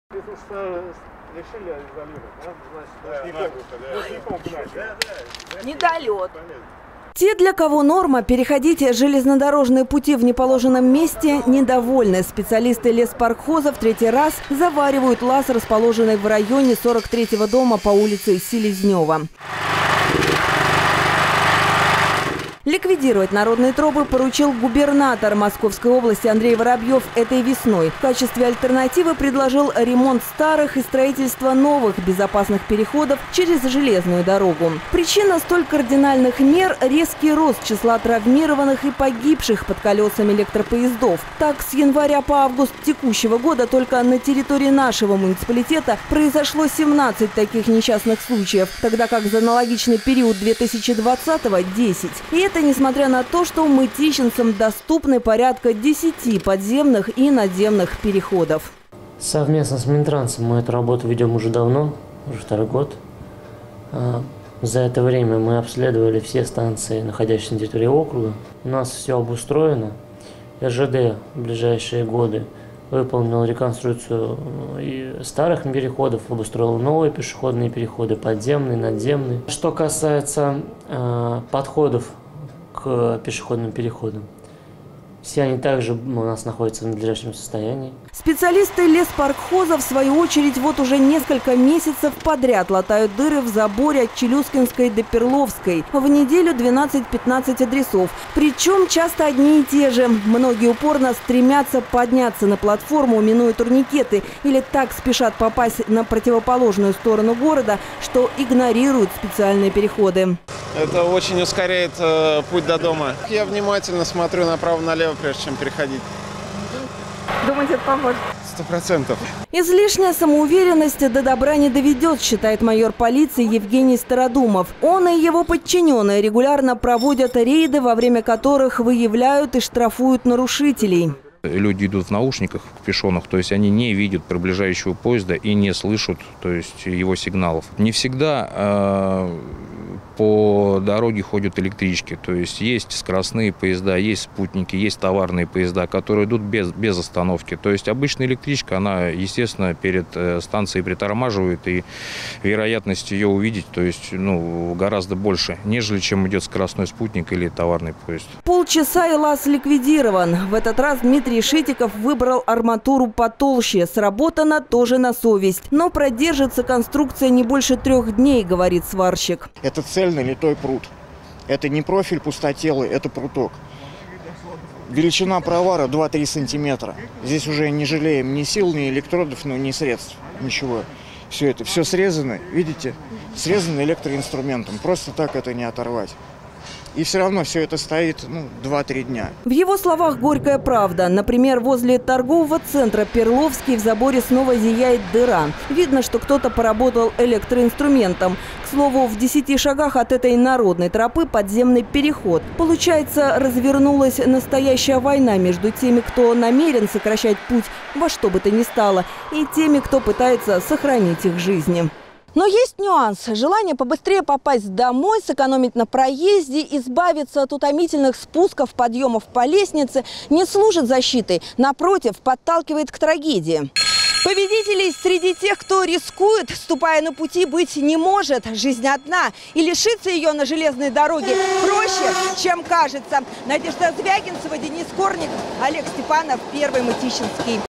Борьба за жизнь продолжается. В округе ликвидируют «народные тропы»
Порядка 200 человек погибли в Московской области под колесами электропоездов в прошлом году. И статистика ползет вверх. На рельсы люди чаще всего попадают по так называемым народным тропам, пытаясь сэкономить деньги или время в пути. Несколько месяцев назад регион принял решение дыры в заборе выявить и ликвидировать. Те, кто не привык пользоваться пешеходными переходами, сдаваться не намерены. (12+)